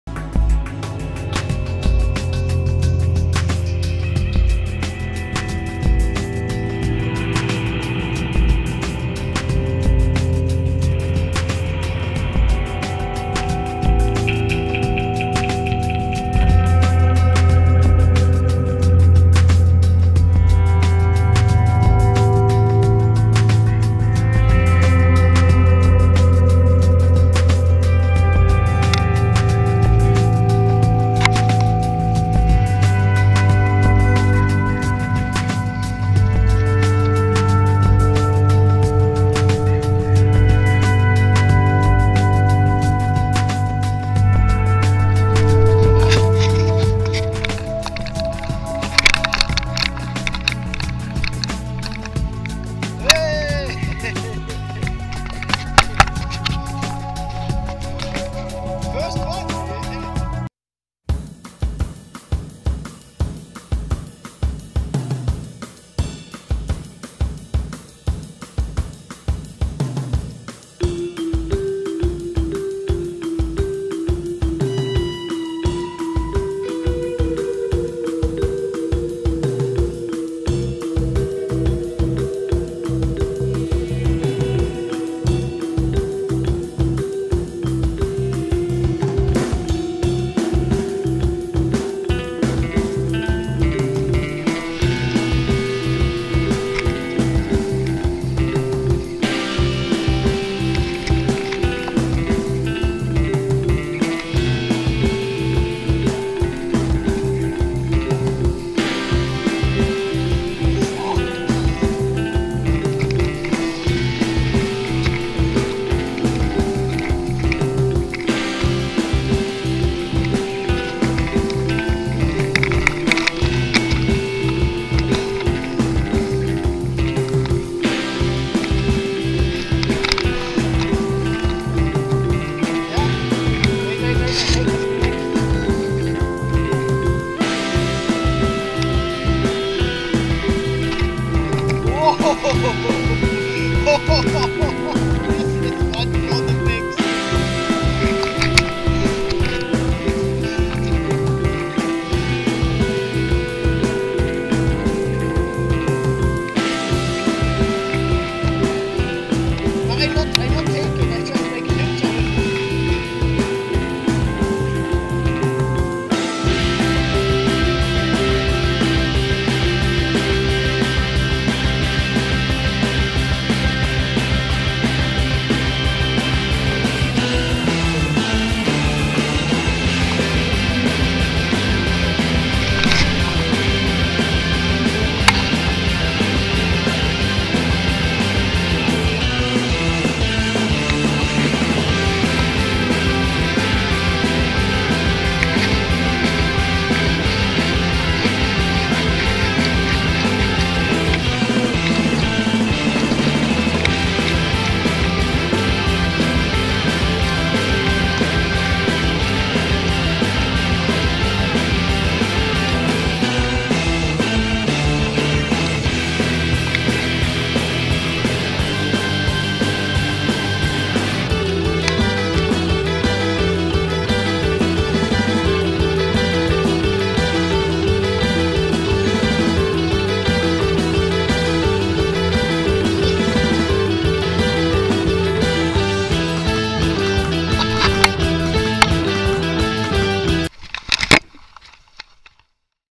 Fishing/Spinning from the shore
Pollachius pollachius in Galway bay
Spinning with metal or plastic lures